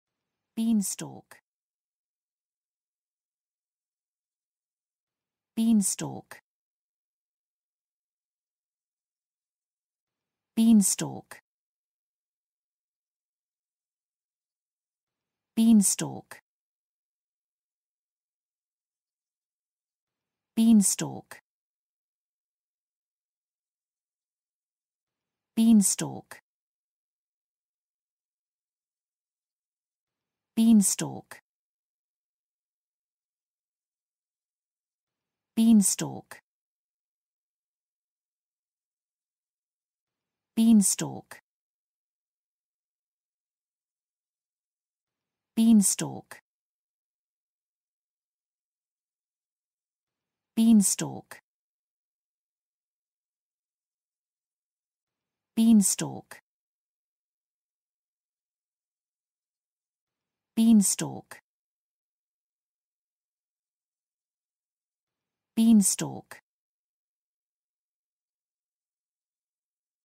beanstalk: How to pronounce beanstalk in english (correct!).Start with B. Learn from me.
Follow me!Learn from me how to pronounce the word beanstalk in english.This video shows you how to pronounce the word beanstalk in english.

beanstalk

n. 豆茎

/ˈbiːnˌstɔːk/

1.   N   名词

the stem of a bean plant 豆茎